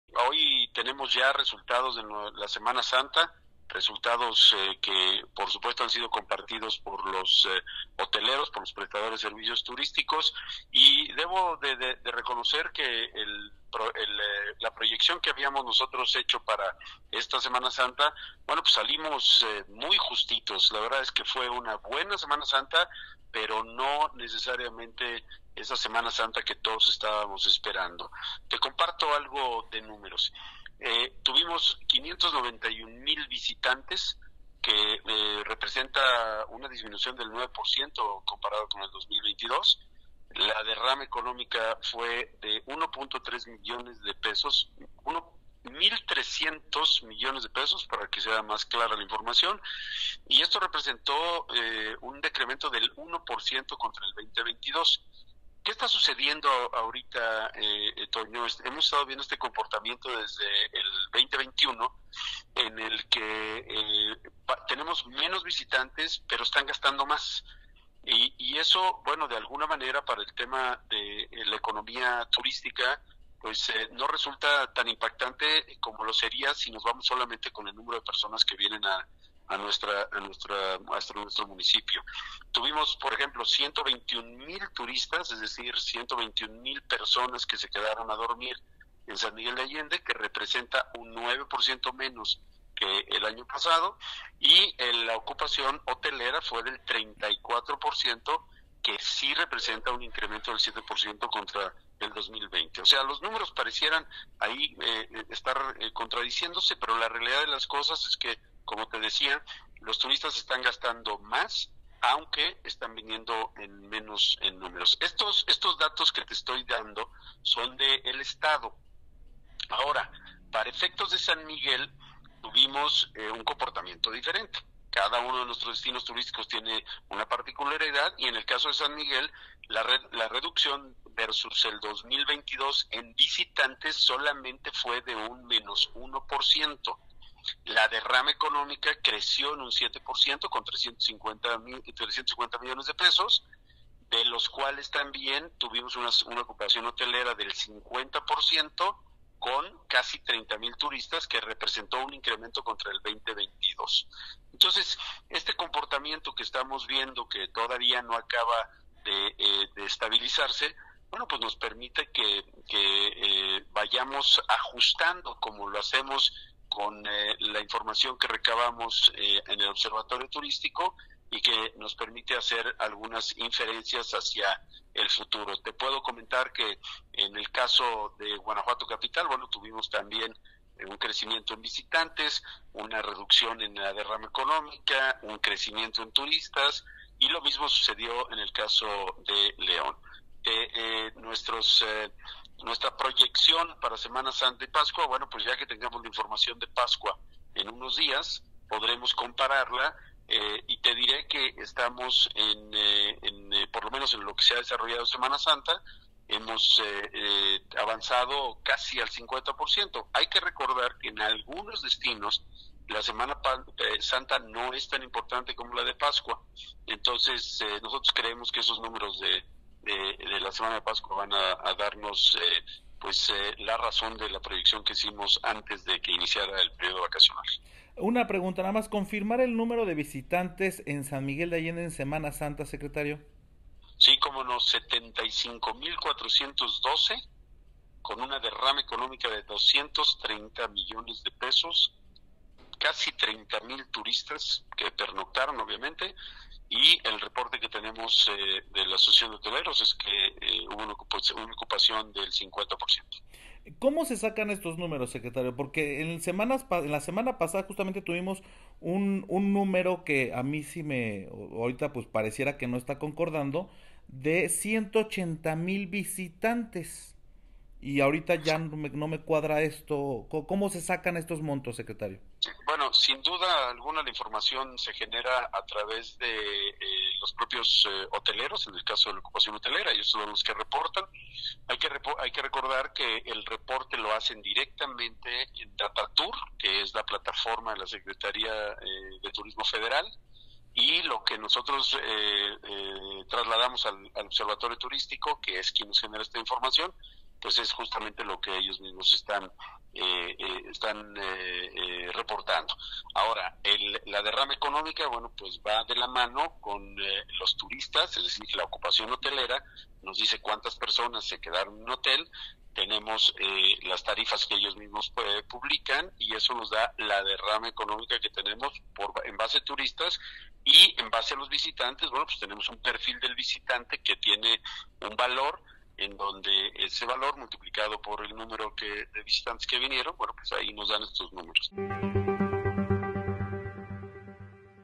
El Secretario de Turismo nos da a conocer la cifras de la derrama que se tuvo en la Semana Santa.
Noticias de San Miguel de Allende, Guanajuato
El Secretario de Turismo del Estado Juan José Álvarez Brunel nos da a conocer la cifras de la derrama económica que se tuvo en la Semana Santa y Semana de Pascua.